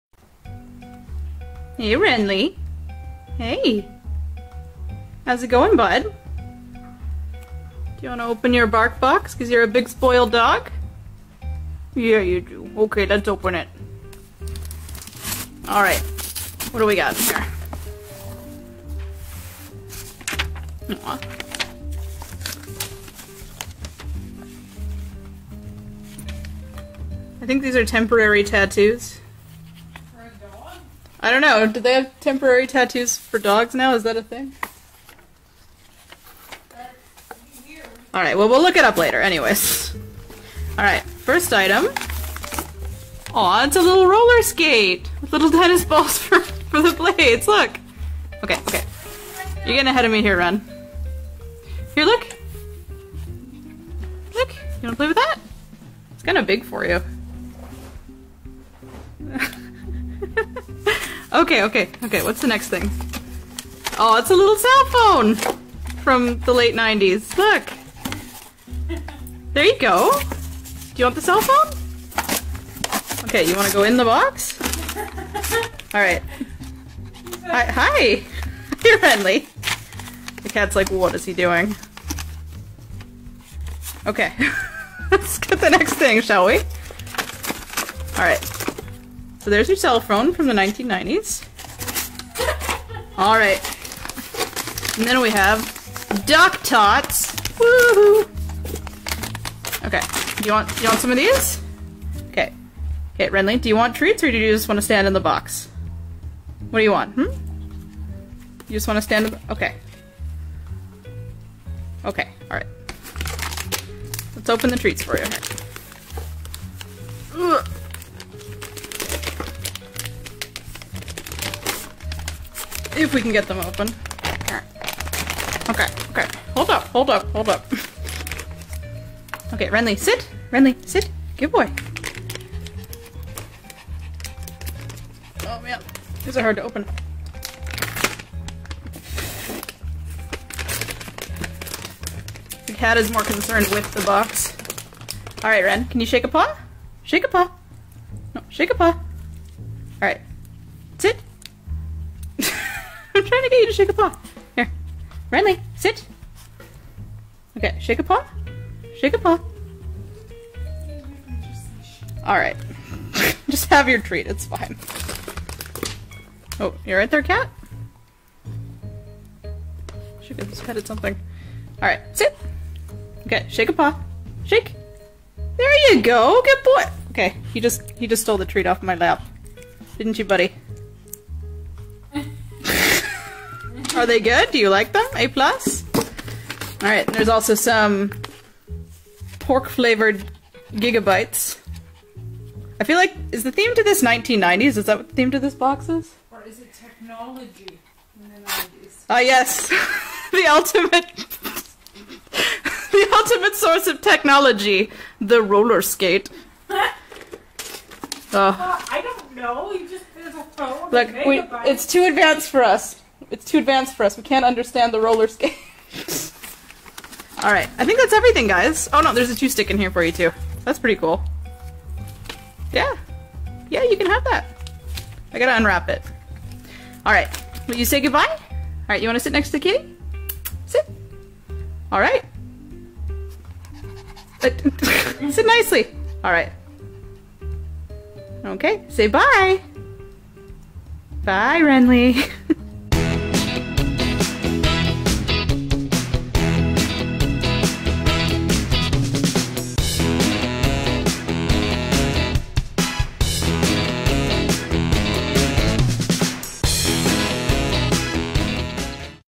Renly the Puppy! - OURS IS THE FURRY?
Meet little Renly! He's half chihuahua, half toy poodle. He's about nine months old here. He weighs about 7 - 8 lbs, but you'd never know it from his big personality.

We got him a bark box for being an awesome little lord.